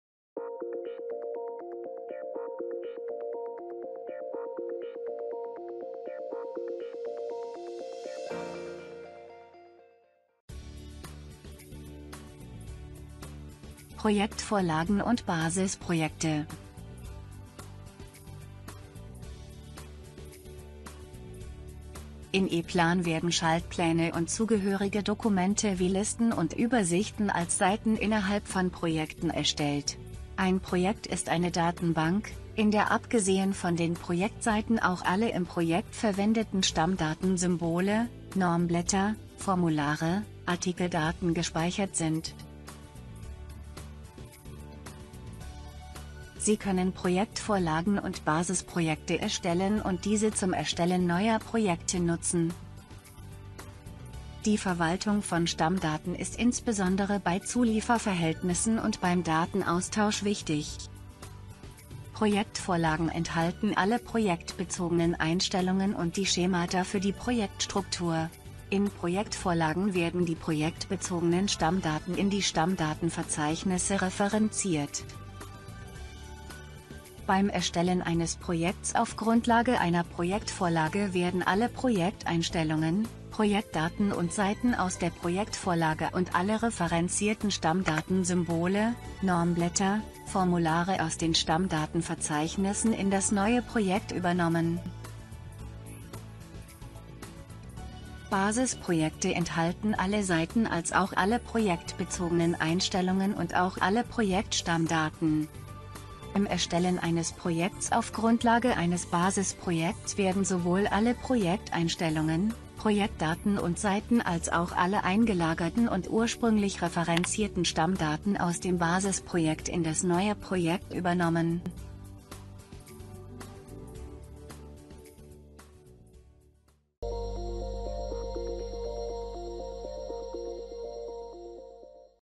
How-to - Basisprojekte
Basisprojekte sind Vorlagen, aus denen Sie neue Projekte erstellen können. Hierbei werden alle Daten des Basisprojekts in das neue Projekt übernommen.